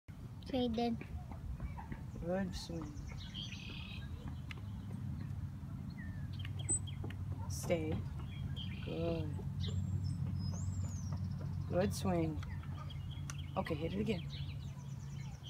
Celtic's Song of Ice and Fire
Freeze enjoying swinging.  She is 4.5 months old American Staffordshire Terrier